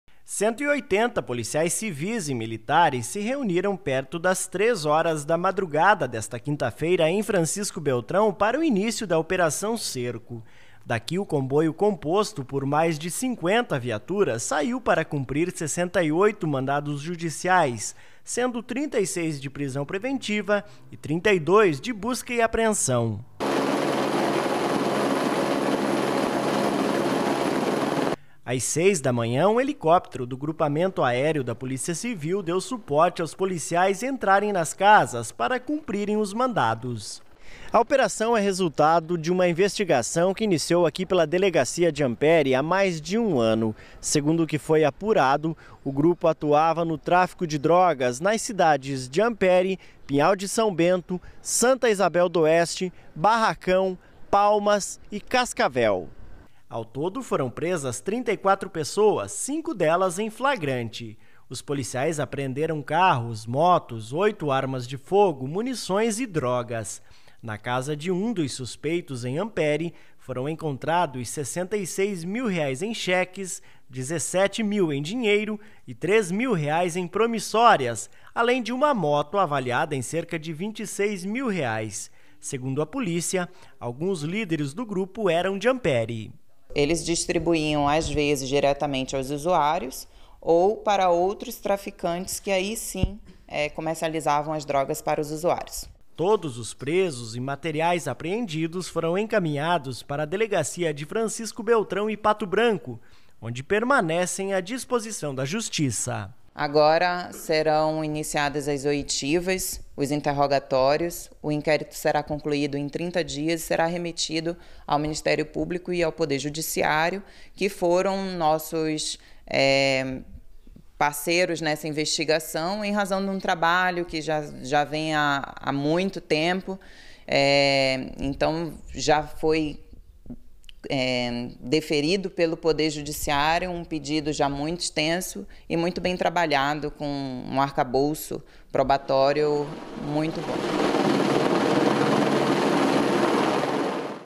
Polícia realiza “Operação Cerco” e prende 34 pessoas
O objetivo foi desarticular um grupo que atuava no tráfico de drogas nas cidades de Ampére, Pinhal de São Bento, Santa Izabel do Oeste, Barracão, Palmas e Cascavel.